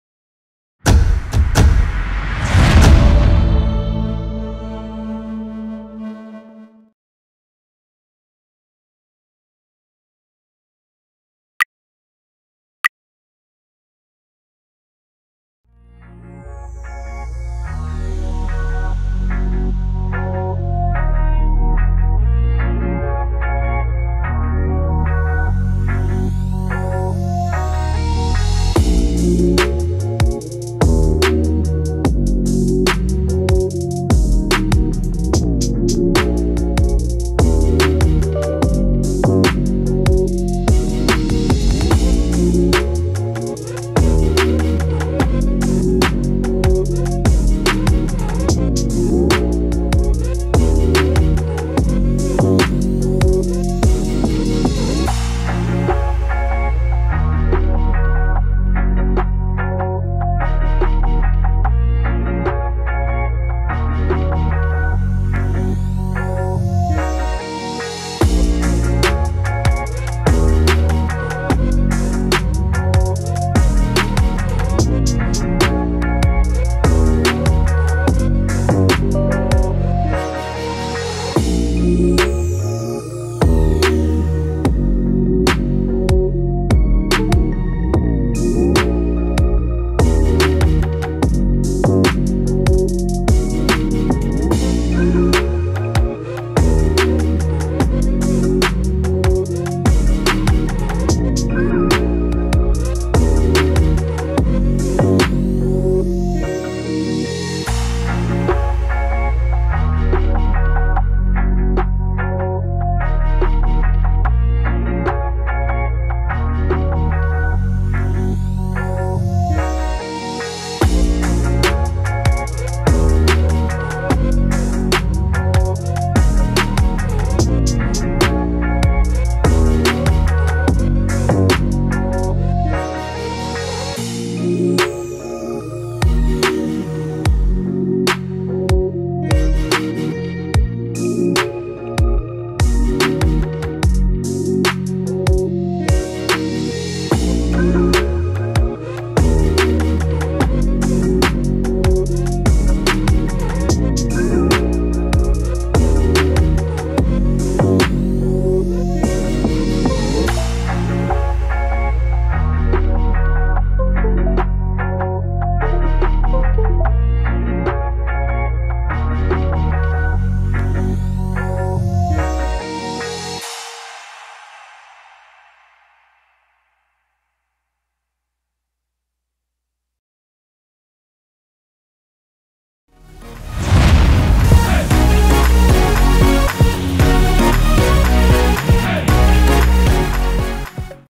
Free TFT MTK Module 3.4 Update, Infinix Hot 8 Lite FRP Bypass Free 1 Click Done By TFT Mtk
Free TFT MTK Tool 3.4 Update,  Infinix Hot 8 Lite FRP Bypass With Free Tool 1 Click Done